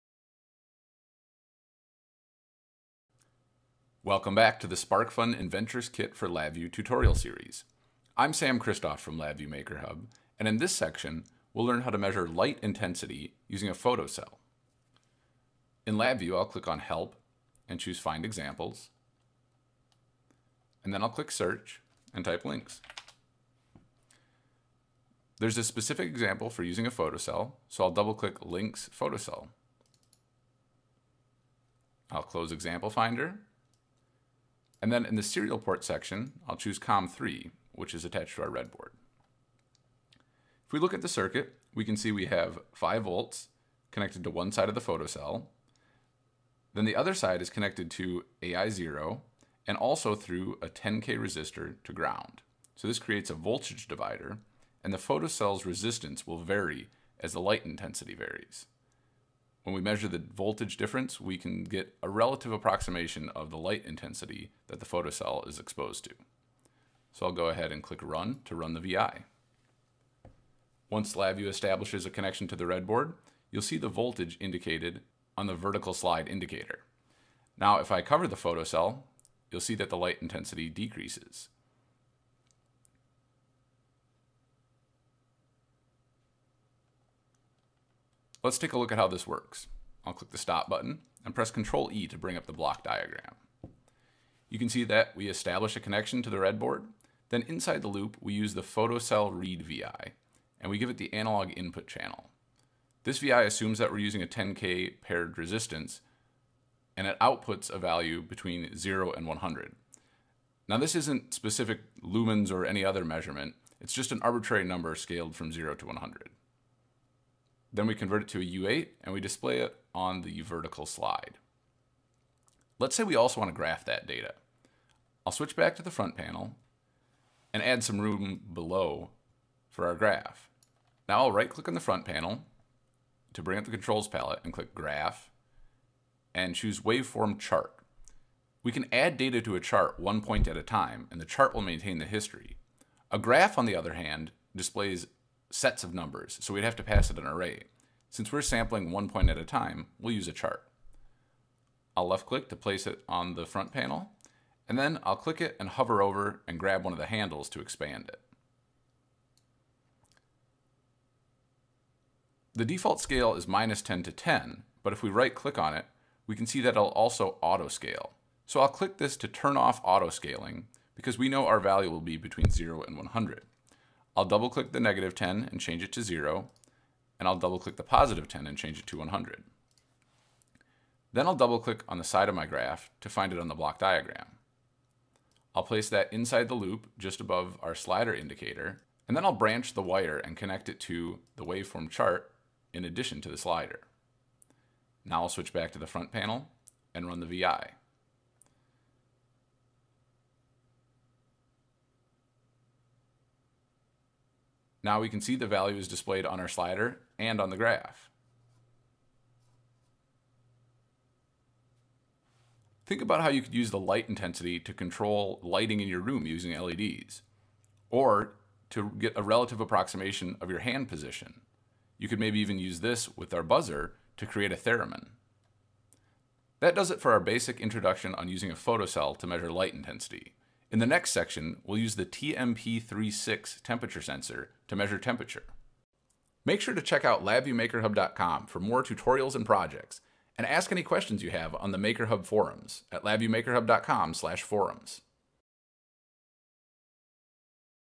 Sparkfun Inventor's Kit for LabVIEW - 10 | Photocell
Photocells vary in resistance with changes in light intensity and are commonly used for detecting ambient light levels for things like automatic car headlights. In this tutorial we'll learn how to measure ambient light and then modify the code to display the light intensity on a graph.